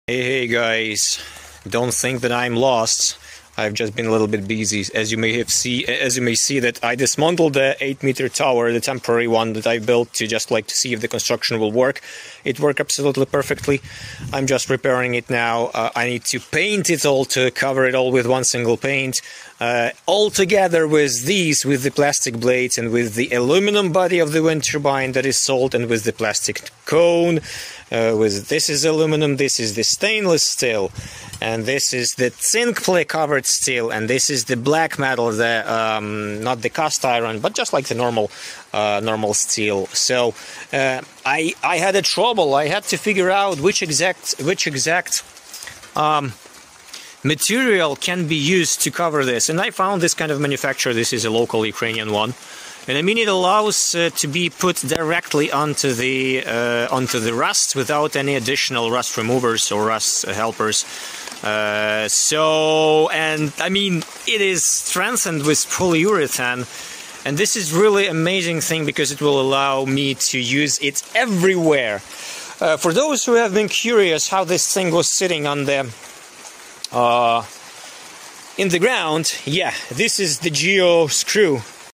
Watch How I Perfectly Built and Dismantled an 8 Meter Wind Turbine Tower!
Curious about how a massive 8 meter wind turbine tower is built and maintained? This clip reveals the entire process from construction to repair, including unique materials and techniques! Perfect for DIY enthusiasts and eco-friendly tech fans. Don’t miss out—share this fascinating engineering journey now!
You may become a sponsor of this channel to gain a little bit of bonuses. Just select the level you like most: